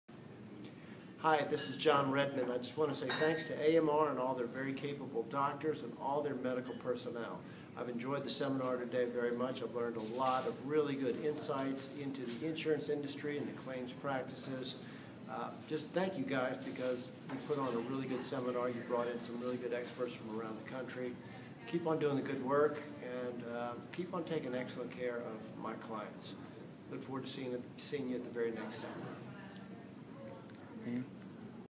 John Redmond
John Redmond shares his appreciation for AMR and their exceptional team of doctors and medical professionals. In this testimonial, he expresses his gratitude for the insightful seminar that provided valuable insights into the insurance industry and claims practices. John commends AMR for bringing in experts from around the country and acknowledges the excellent care they consistently provide to his clients. If you're looking for valuable information on the insurance industry or high-quality medical services, AMR is the go-to choice.

Are you looking to elevate your business or considering selling? and embark on a path to success today!